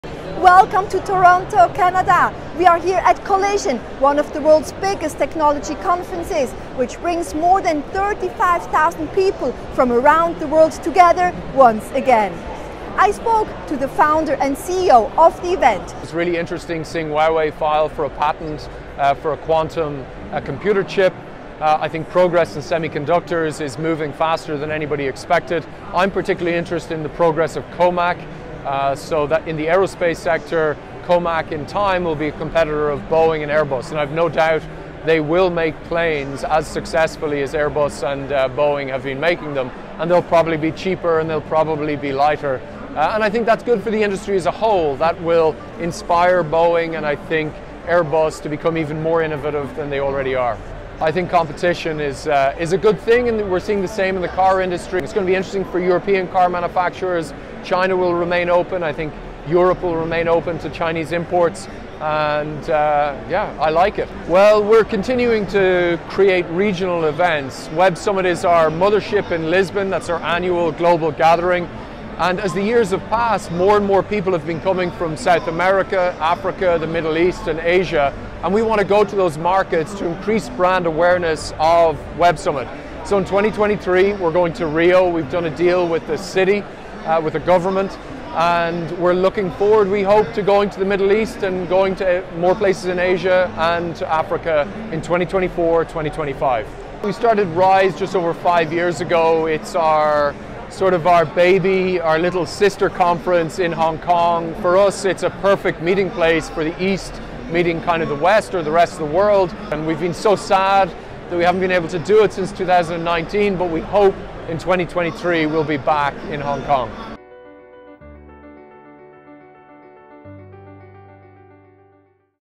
GLOBALink | CEO of Web Summit and Collision says China to leapfrog in technology development
China is progressing rapidly in the development of aerospace, quantum computing and electric vehicles, which lead to more innovative competition, said Paddy Cosgrave, the founder and CEO of Web Summit and Collision